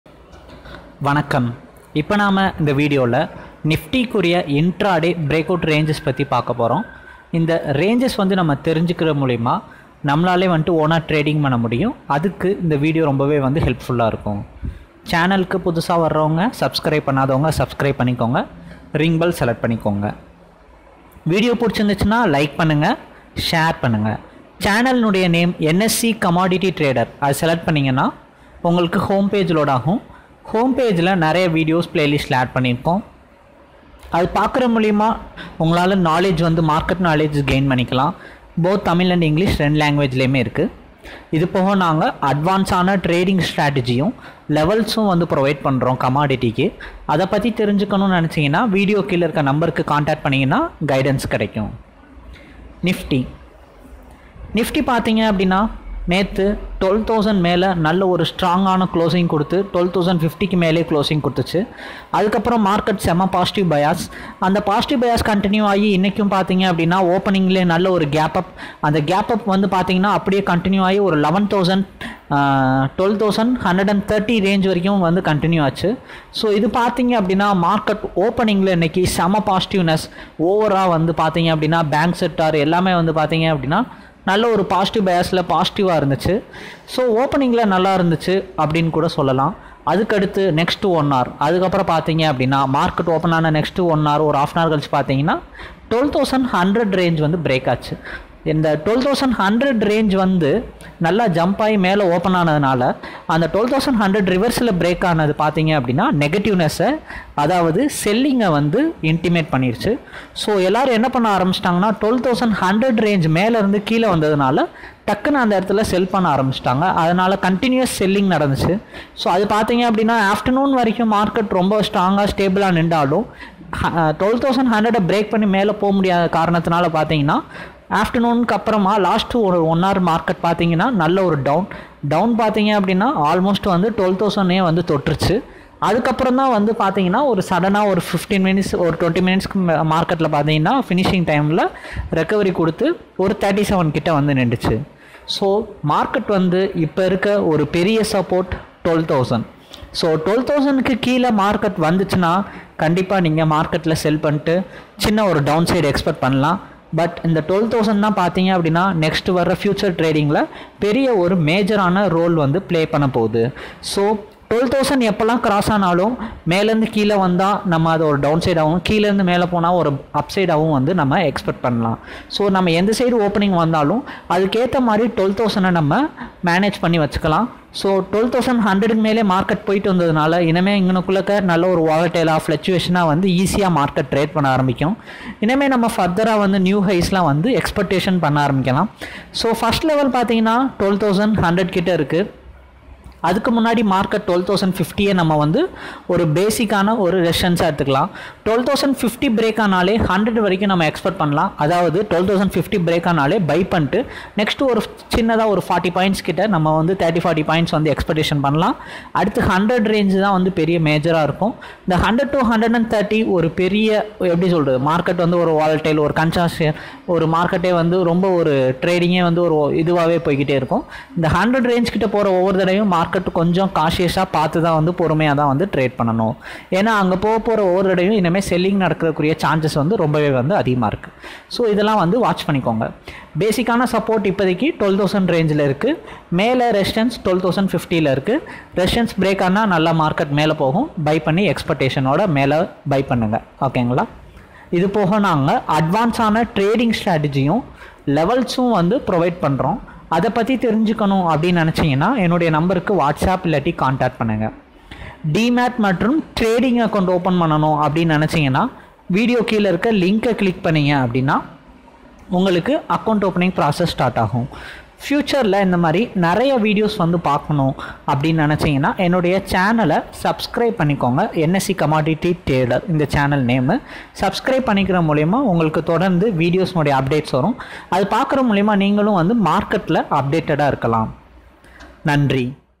Nifty Technical View | 3rd Doji Week | Intraday Breakout Range
Open account  by clicking LINK - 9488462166

This Video about NIFTY Breakout Range for Intraday Trading.

Nifty need further breakout at 12050 resistance
12000 act as support